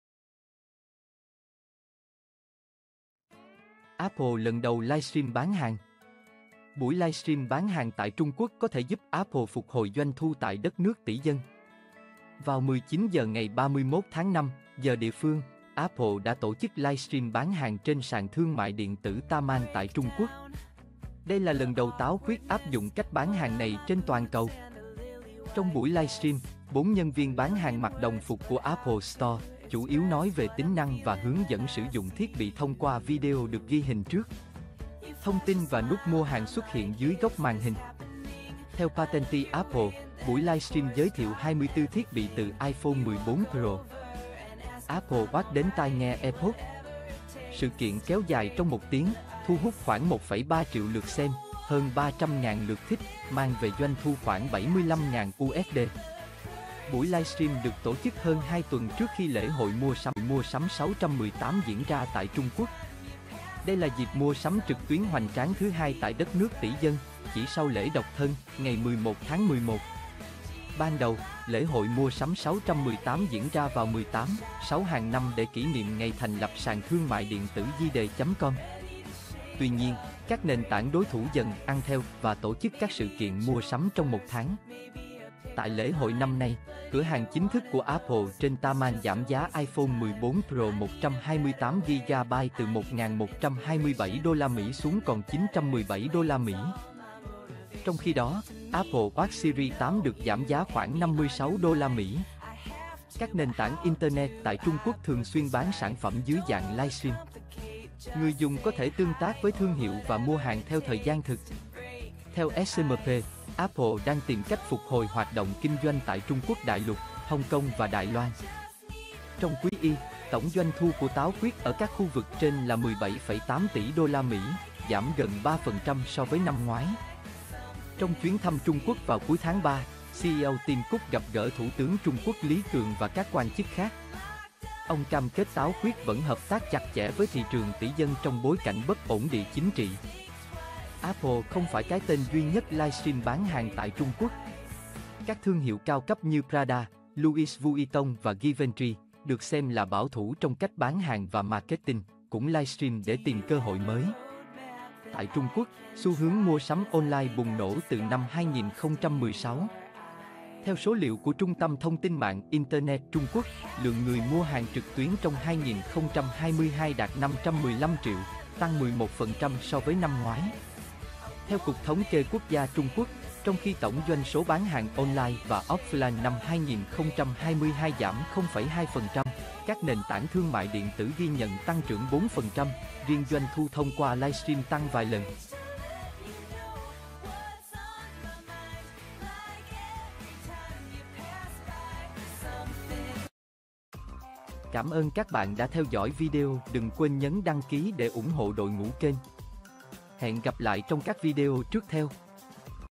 Apple lần đầu livestream bán hàng.
Chào các bạn, vừa rồi có tin tức Apple lần đầu livestream bán hàng tại Trung Quốc, mời các bạn cùng theo dõi thông tin nhé.